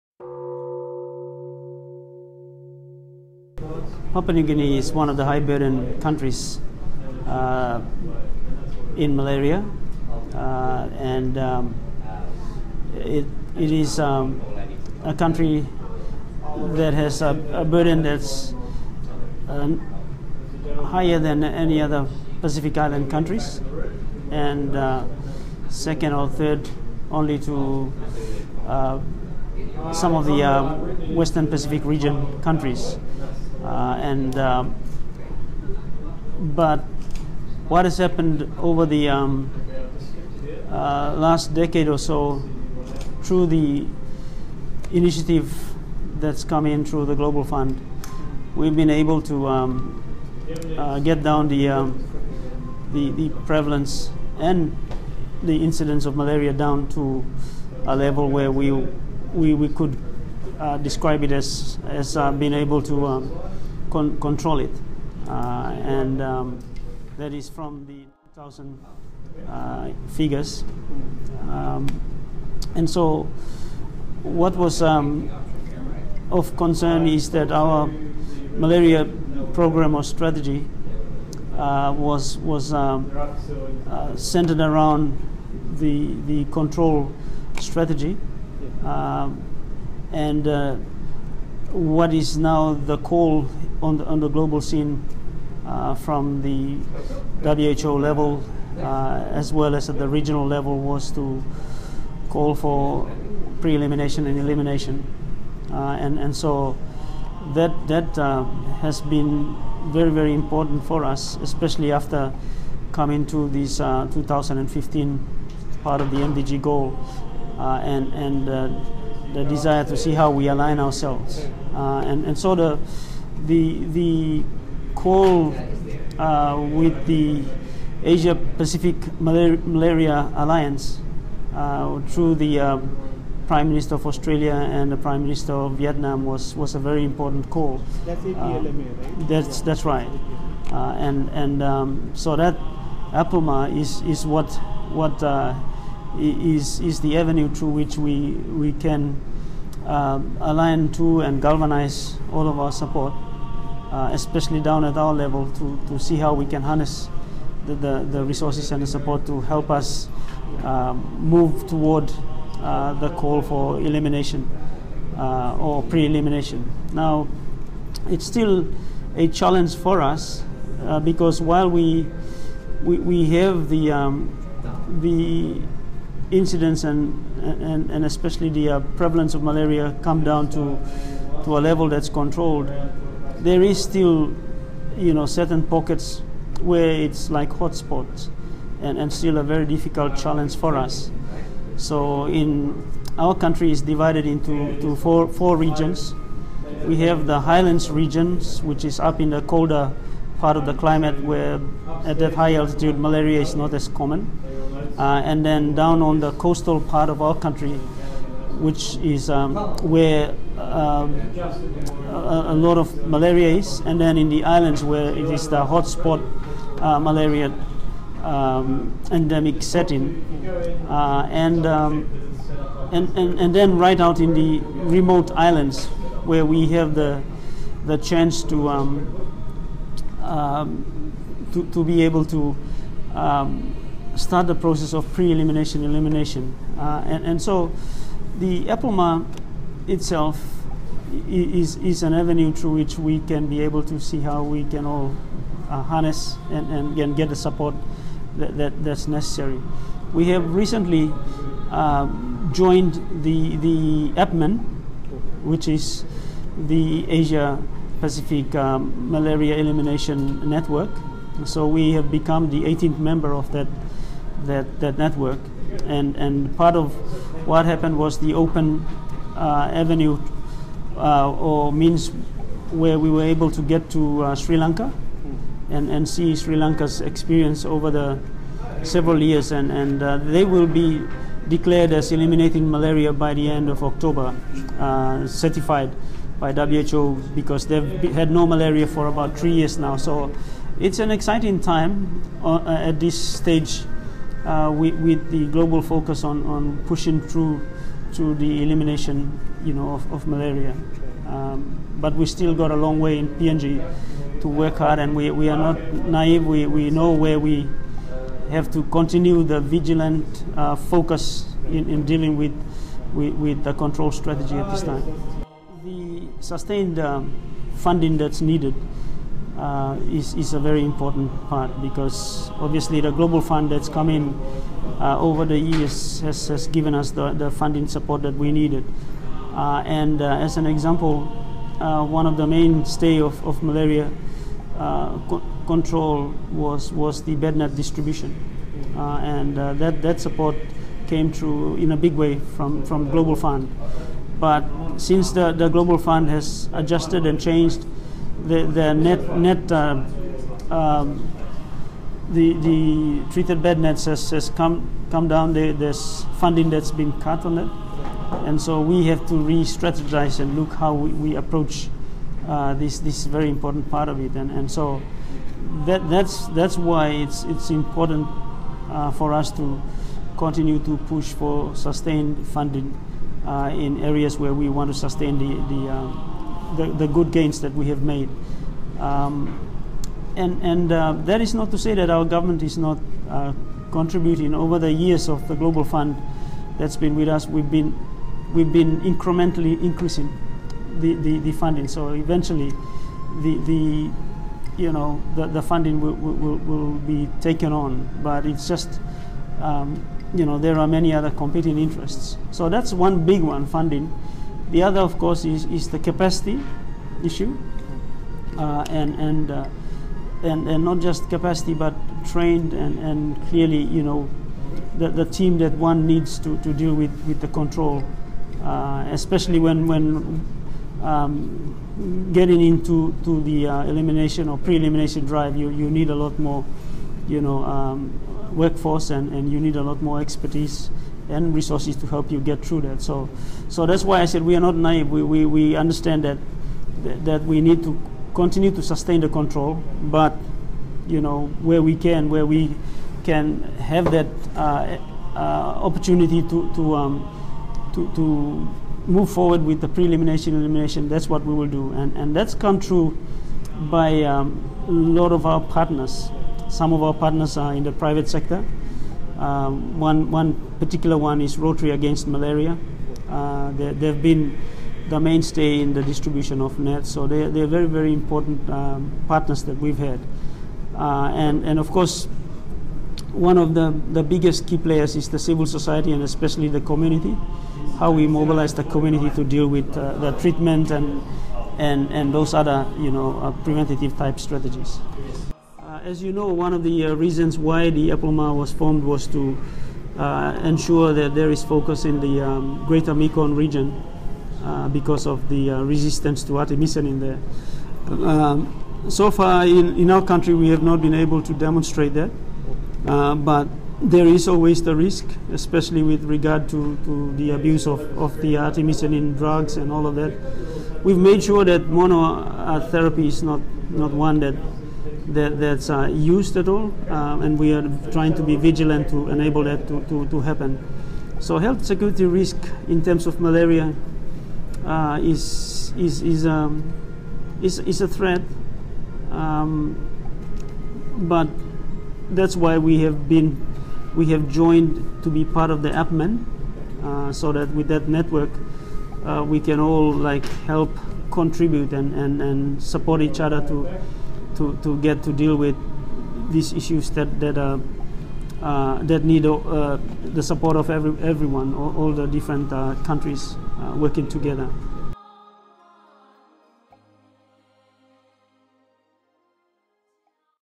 Focus: Fight against malaria in Papua New Guinea: A long way to go for elimination!
Fight against malaria in Papua New Guinea (PNG) has had some successes but a long formidable road remains ahead for PNG to meet the regional goal of malaria elimination by 2030.

Dr Paison Dakulala, Deputy Secretary of Department of Health, Papua New Guinea (PNG) spoke with Citizen News Service (CNS) in Sydney on the sidelines of the 5th Asia Pacific Region Conference of the International Union Against Tuberculosis and Lung Disease (The Union).

Focus, is a CNS video series on current global health and development priorities. This is episode 1.